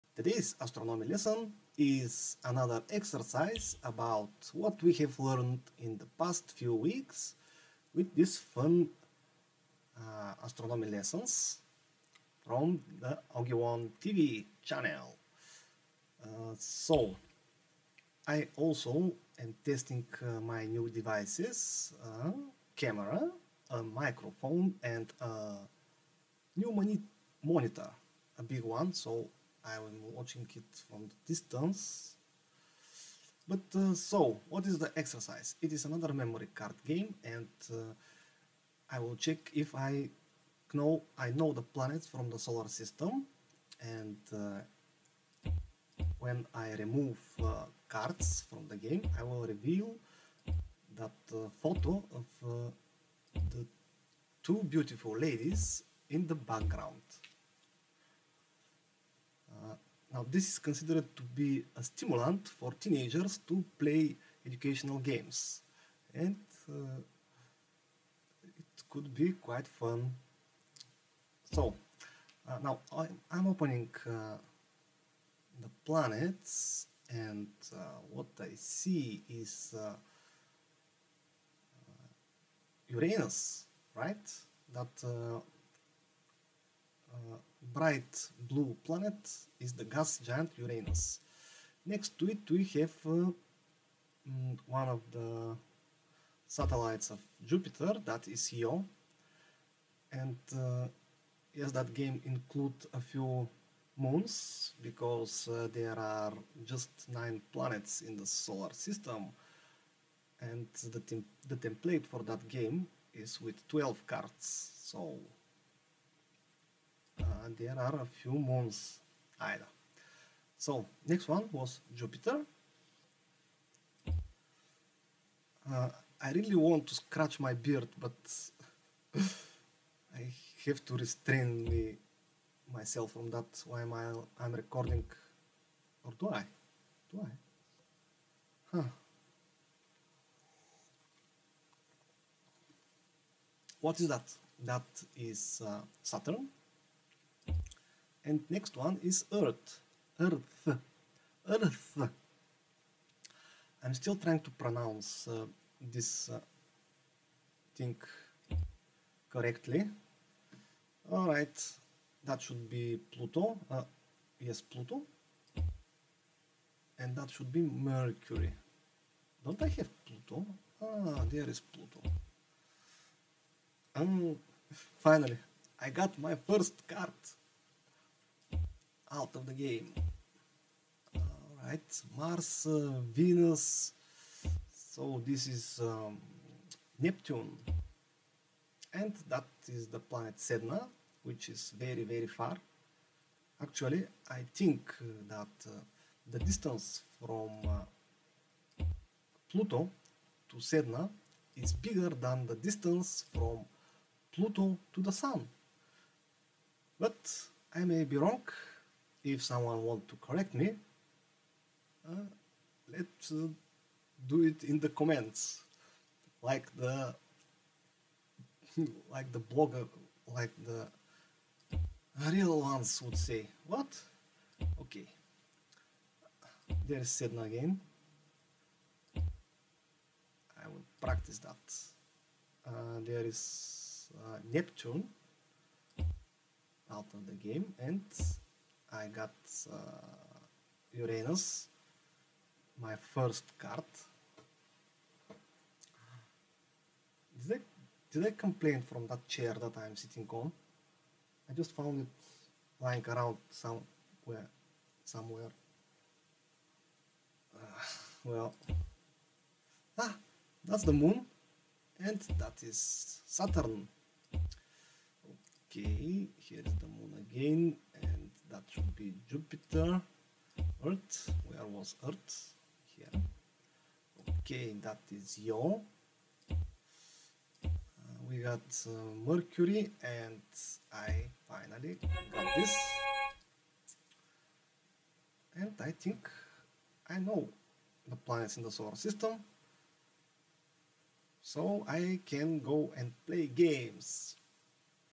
Planets Practice - Interactive Astronomy Lesson 🪐🎥🎓
Fun educational video with memory exercise over the planets in the Solar System. 10 planets and 2 moons in a fun online memory cards game. Free online astronomy video lesson for 6 grade.  Interactive Video Lesson Astronomy.